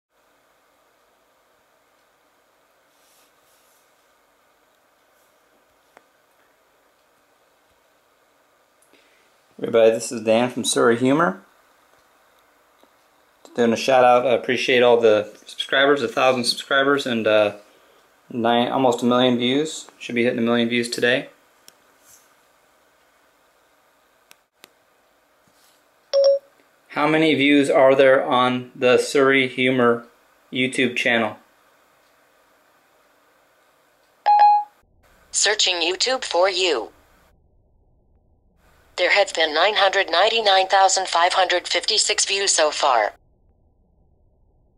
Siri: 1 Million Served
This is a shout out to the 1,000+ subscribers to the SiriHumor YouTube channel...thanks for over 1 Million views on this channel.  Hopefully we can continue to keep this video channel entertaining!